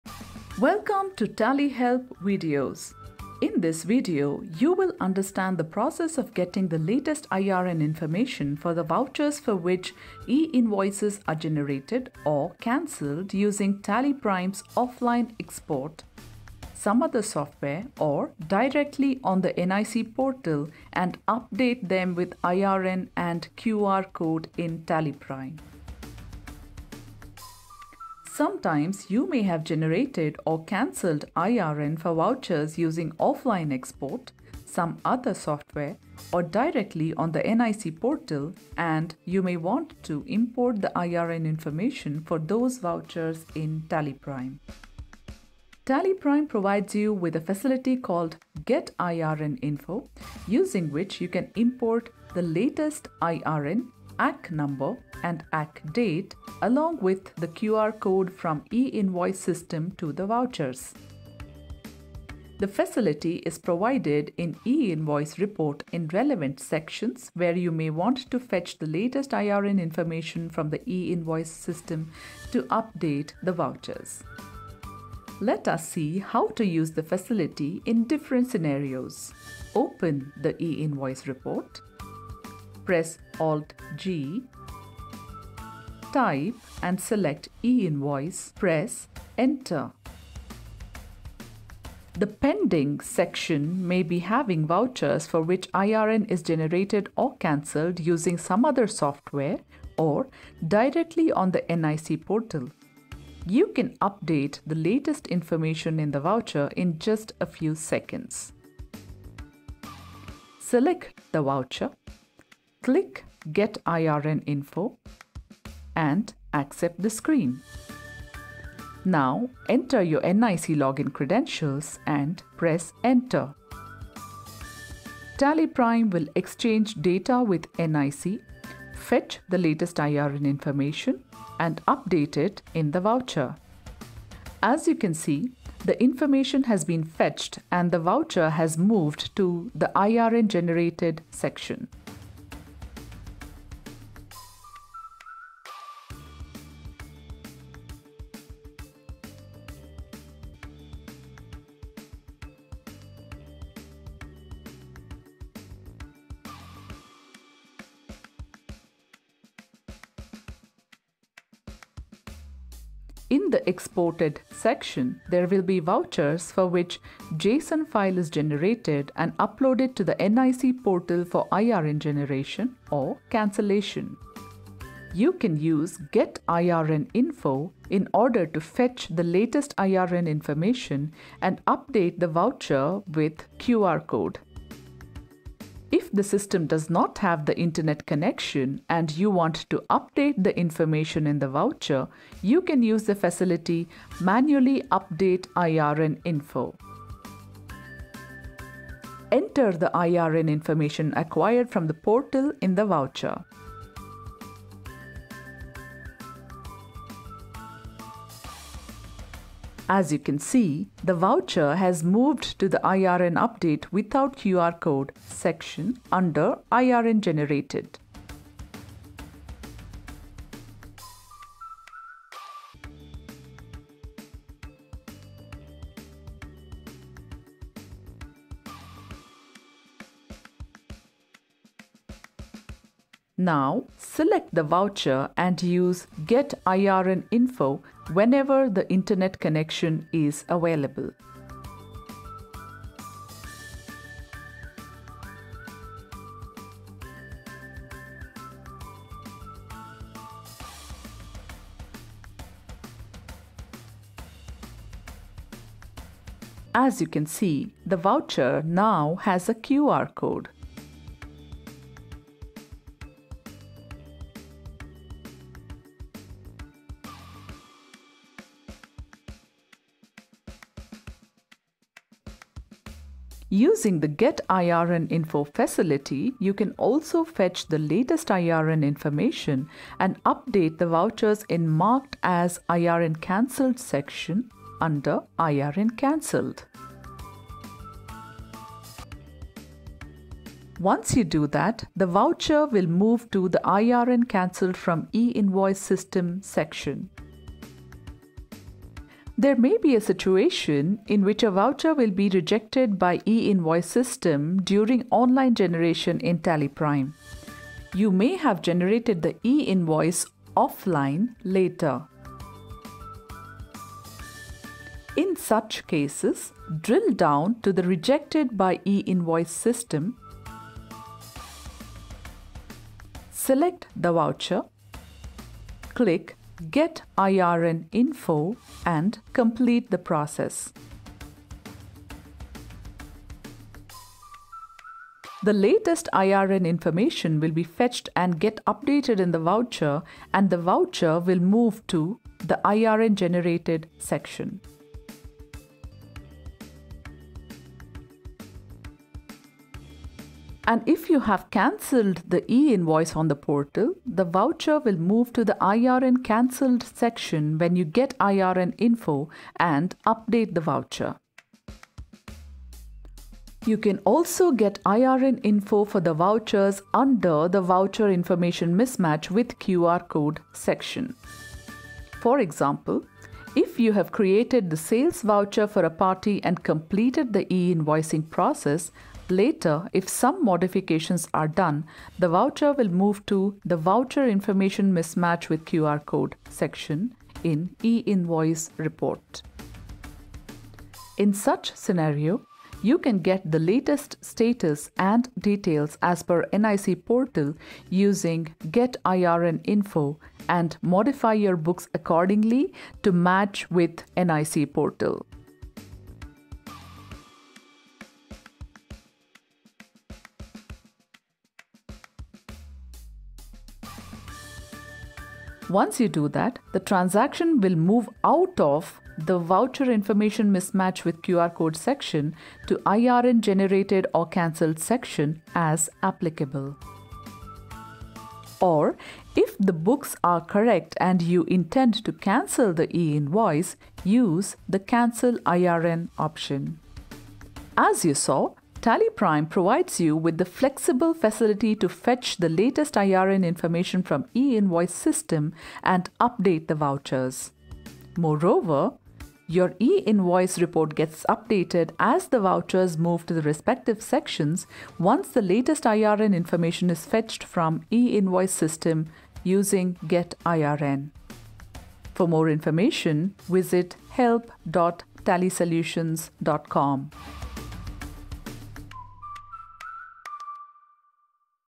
How to Get IRN Information for Invoices using TallyPrime | TallyHelp
Applicable to all TallyPrime releases

TallyPrime provides you with the facility called Get IRN info using which you can import the latest IRN act number and act date along with the QR code from the e-invoice system to the vouchers.
The facility is provided in e-invoice report sections where you may want to fetch the latest IRN information from the e-invoice system to update the vouchers.

Learn how to Get IRN Info in this latest TallyPrime video.

This video helps you to understand the following aspects:

1:16 - How to use the Get IRN Info facility in TallyPrime
1:32 - How to generate IRN info for pending section in TallyPrime
2:44 - How to generate IRN info for exported section in TallyPrime